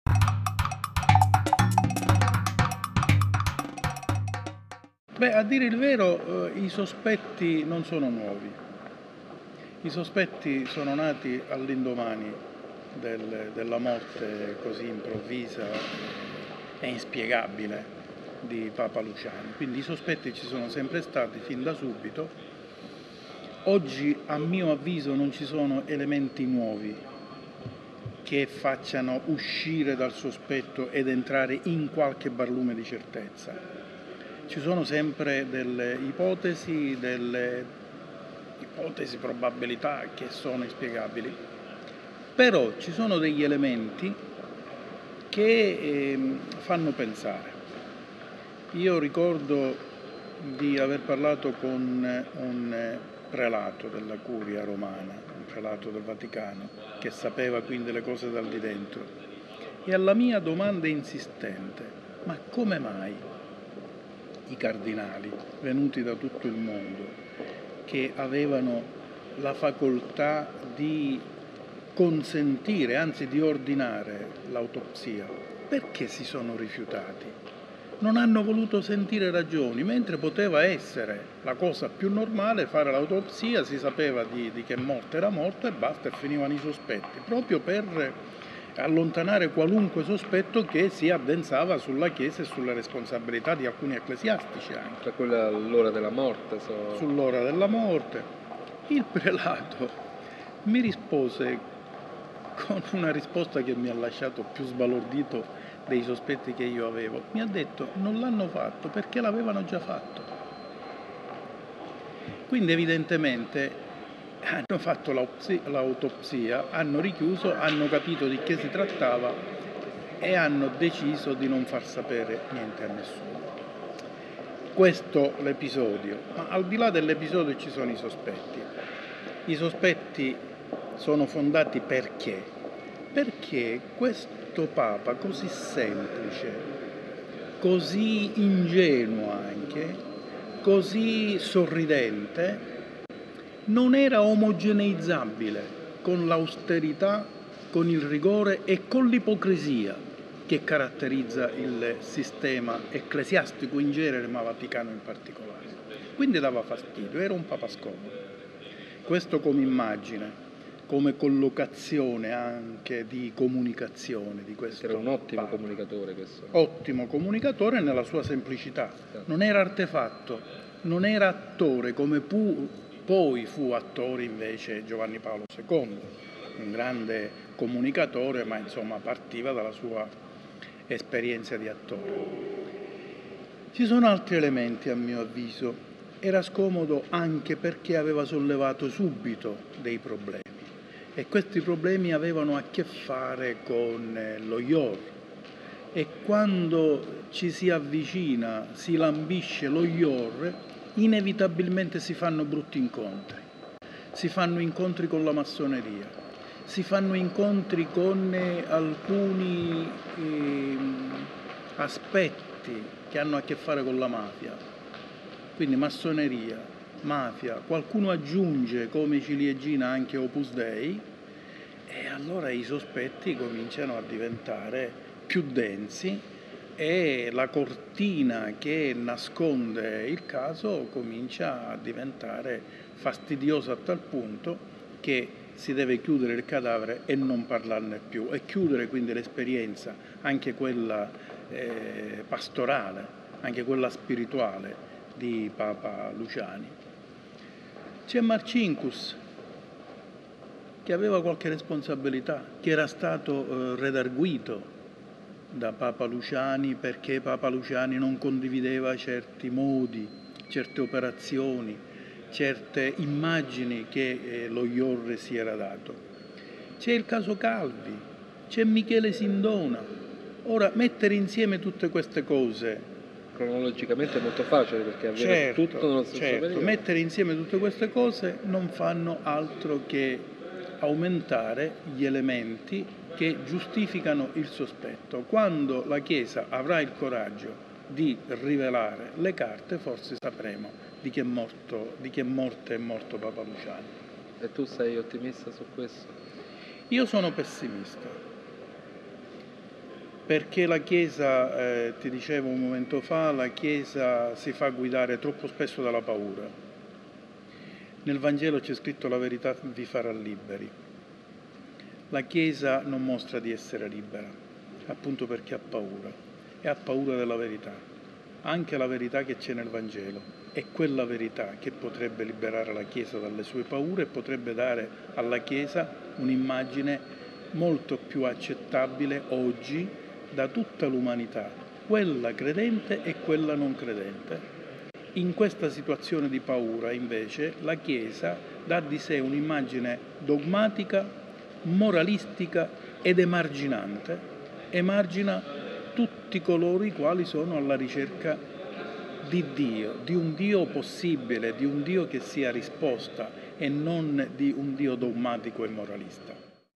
Giovanni Avena: la morte di Papa Luciani
Giovanni Avena, direttore di Adista: agenzia di stampa cattolica, parla del mistero che circonda la morte di Papa Luciani.

Giovanni Paolo I, nato Albino Luciani (Canale d'Agordo, 17 ottobre 1912 - Città del Vaticano, 28 settembre 1978), fu il 263° Papa della Chiesa Cattolica Apostolica Romana e successe il 26 agosto 1978 a Papa Paolo VI.
Il suo pontificato fu tra i più brevi della storia: la morte avvenne doposoli 33 giorni dalla sua elezione al soglio di Pietro.
In questa intervista Giovanni Avena ripropone molti dei dubbi sulle cause della morte di Papa Luciani, e in particolare rivela alcuni retroscenas ull'autopsia che, nei comunicati ufficiali non venne mai effettuata, ma che secondo le informazioni in mano al direttore di Adista fu fatta immediatamente dopo il decesso del pontefice e poi mantenuta segreta.
L'improvvisa morte di papa Giovanni Paolo I, avvenuta nella notte del 28settembre 1978, ha dato vita a tutta una serie di ipotesi e speculazioni.